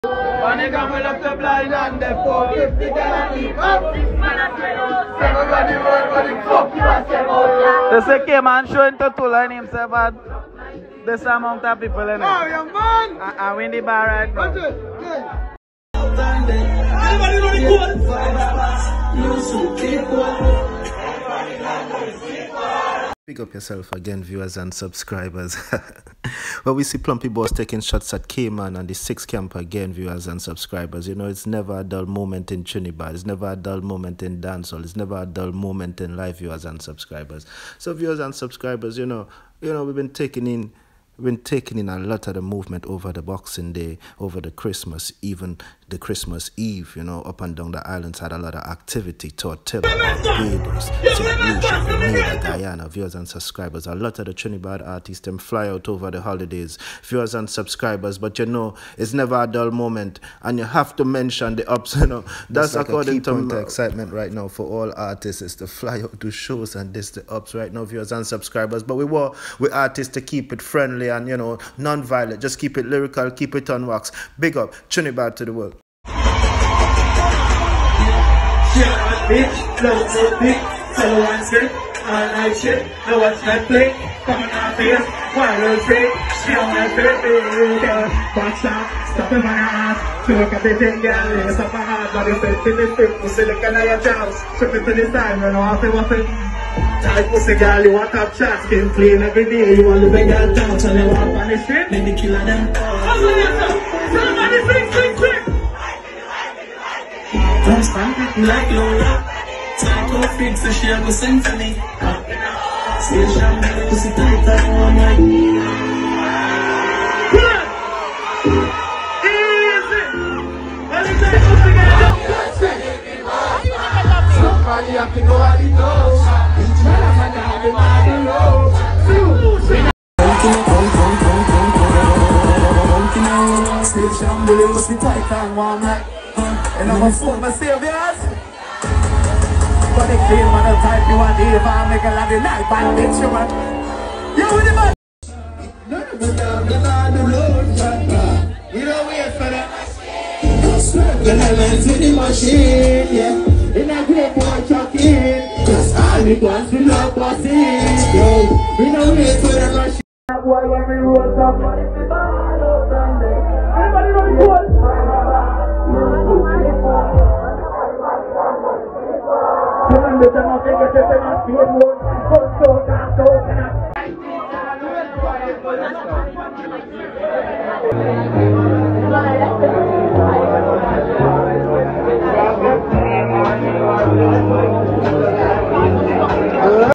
Plumpy Boss Diss Kman 6 in St.Lucia after concert Dec 2023.
Plumpy Boss says Kman 6 show in Tortila British Virgin Islands was low in attendance.